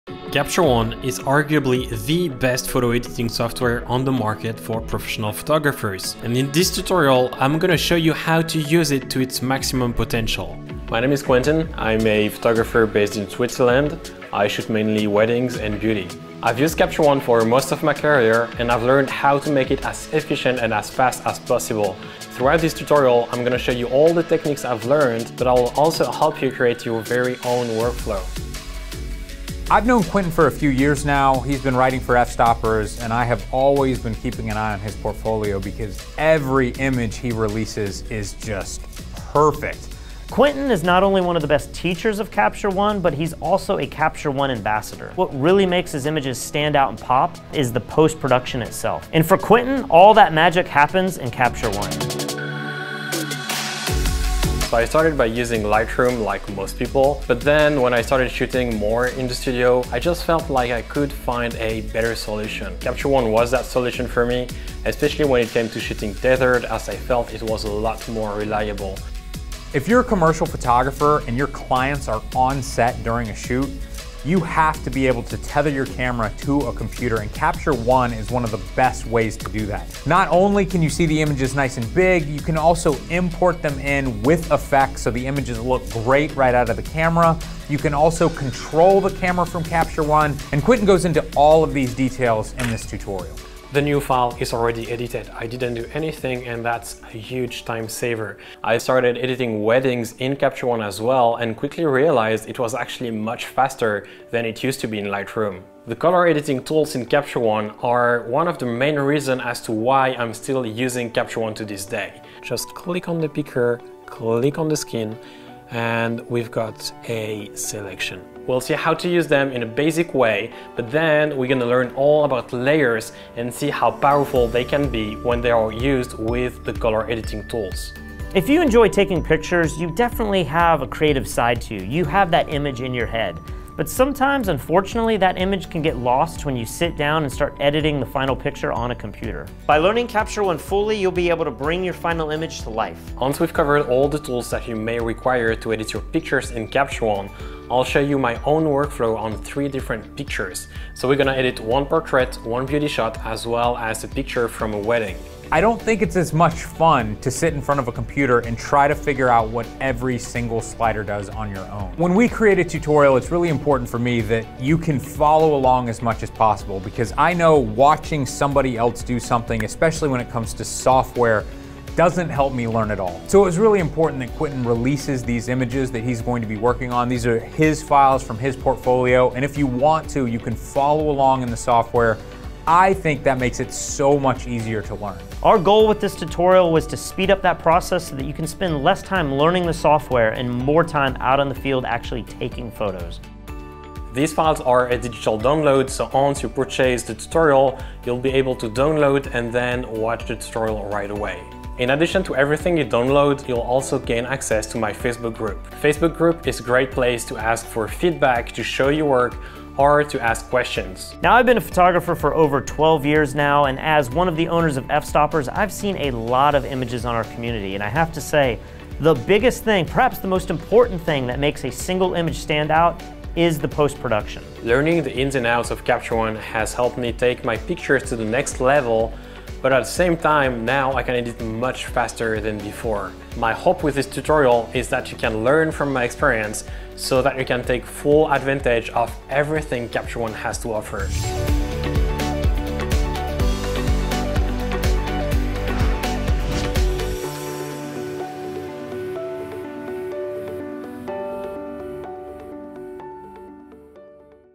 The Complete Capture One Editing Guide Promo
Fstoppers has created a 5 hour tutorial with Quentin Decaillet teaching you everything you need to know about using Capture One efficiently and effectively.